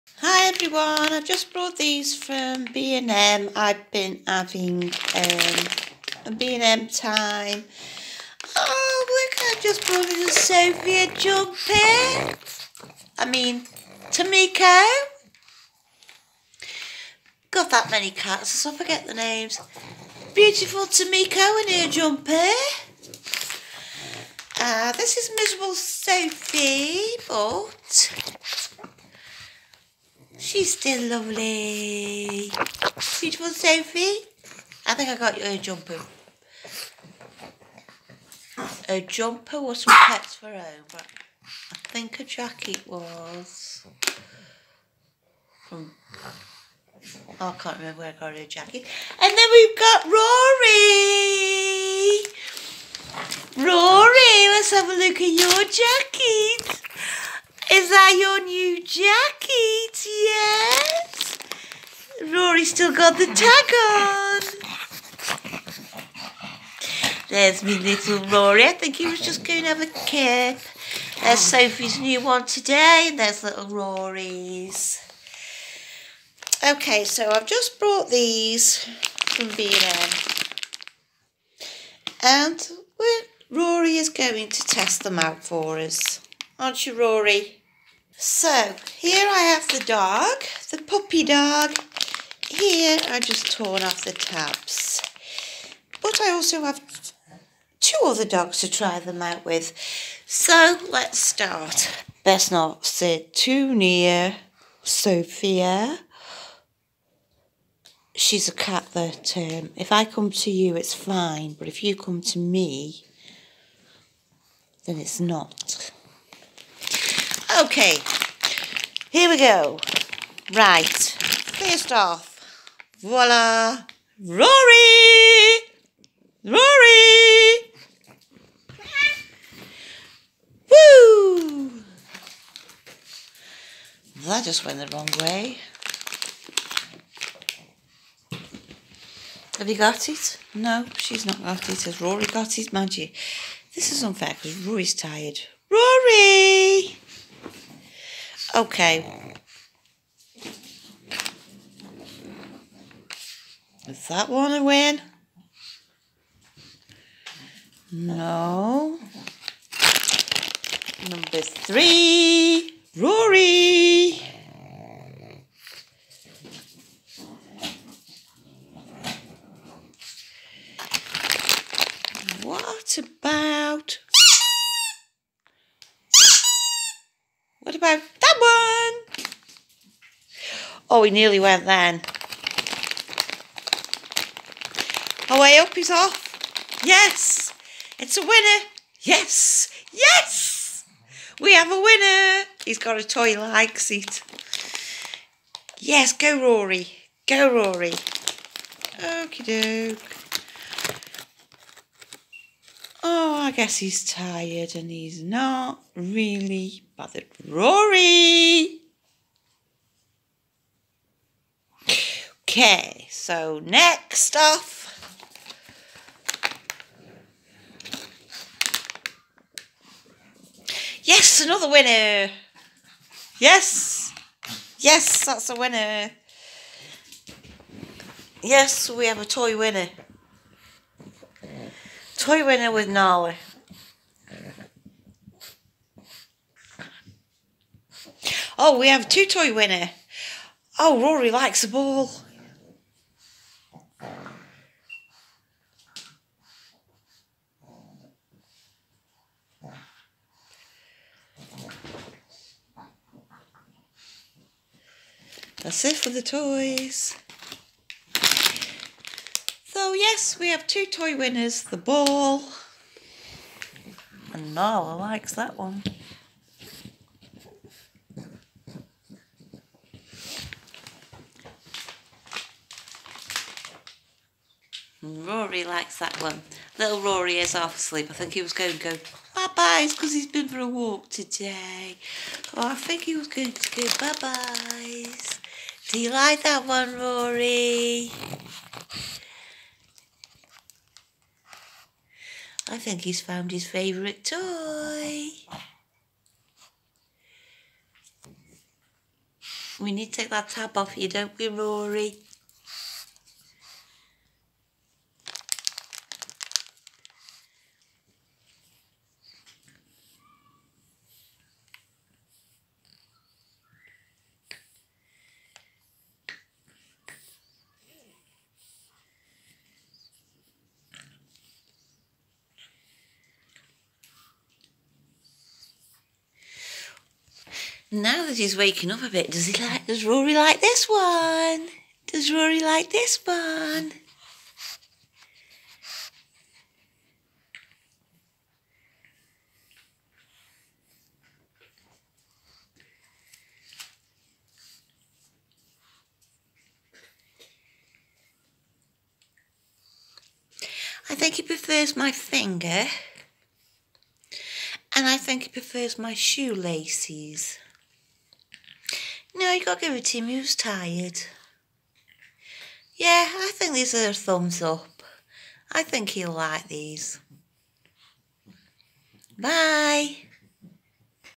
RORY TRIES PUPPY TOYS FROM B&M
Rory french bull dog. Ties out new puppy toys